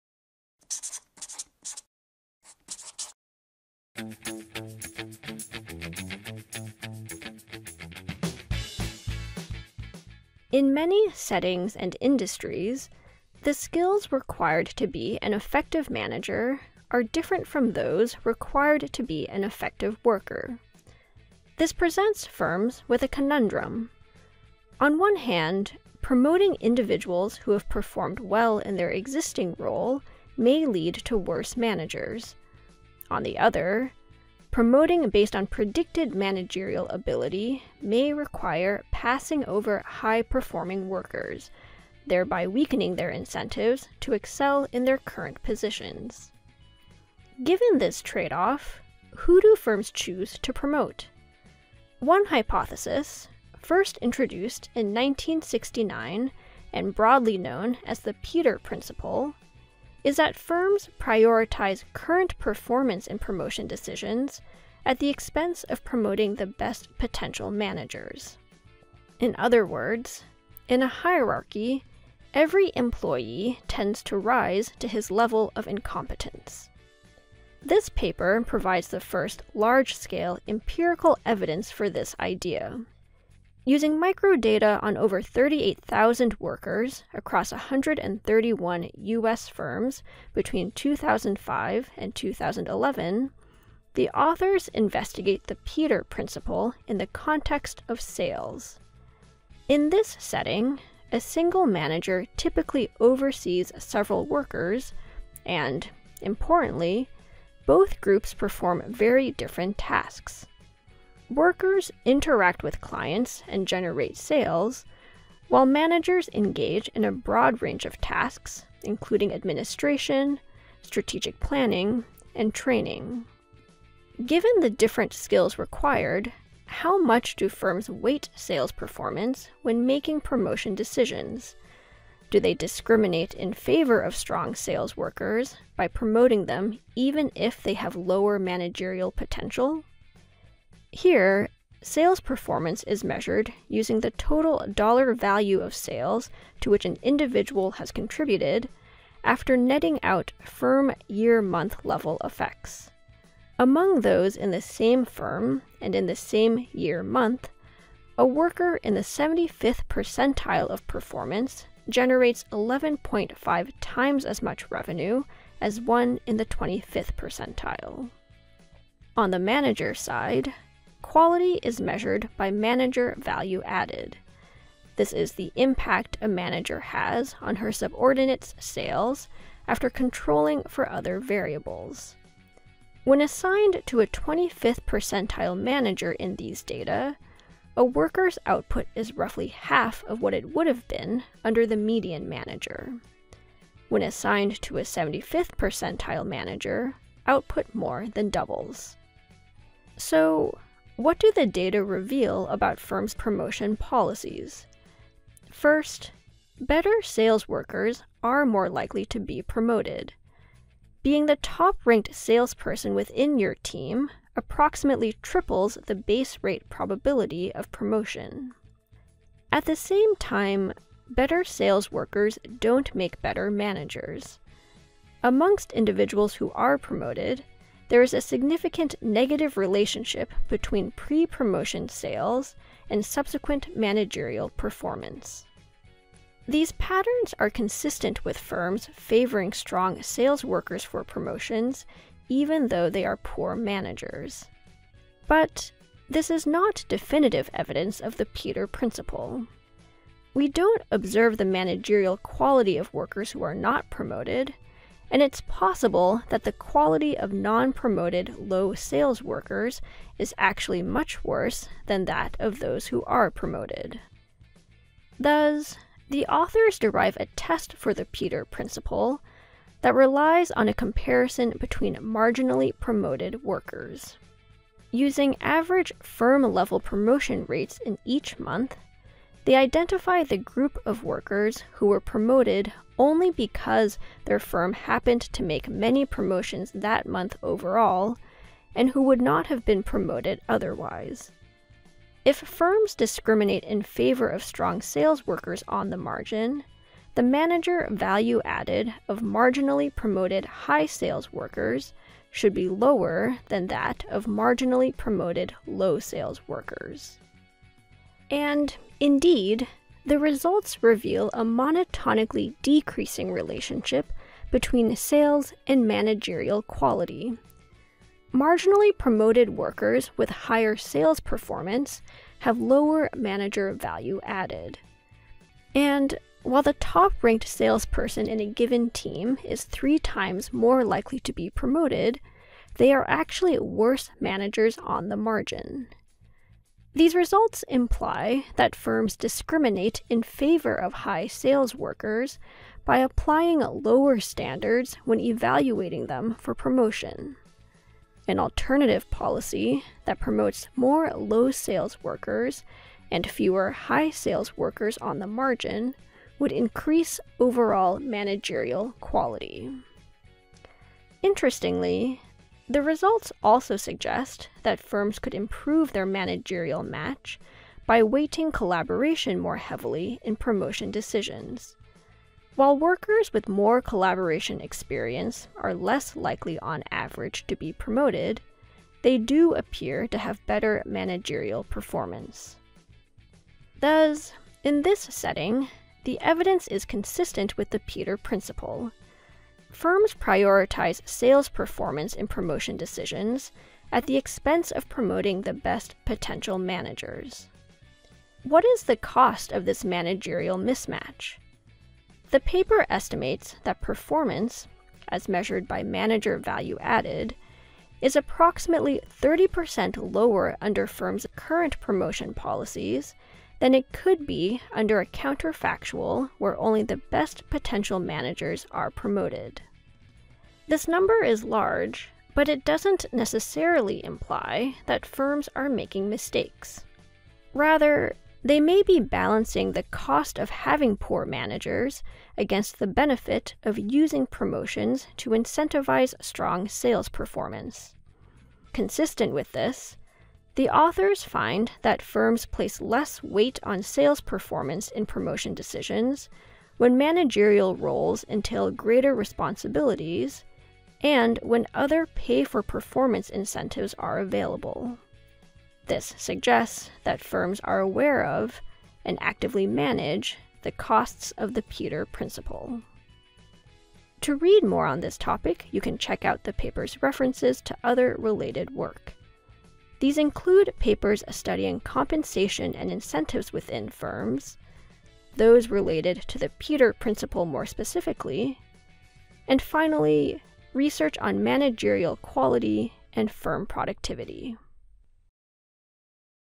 Do Firms Promote the Right People?
Alan Benson, Danielle Li, and Kelly Shue. 2019. “Promotions and the Peter Principle.” The Quarterly Journal of Economics 134 (4): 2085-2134.

When effective workers are not necessarily effective managers, who do firms choose to promote?